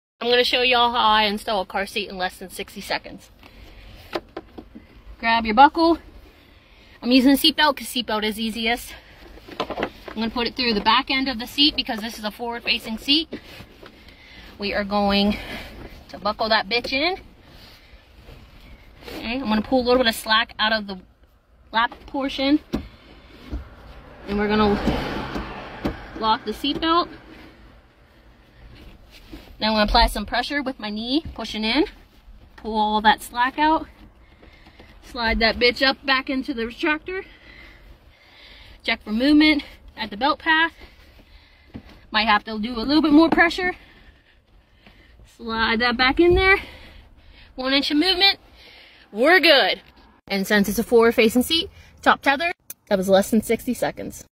Forward Facing car seat install
After many people have complained that they have such a hard time installing a car seat, here I demonstrate how I install a forward facing seat in less than 60 seconds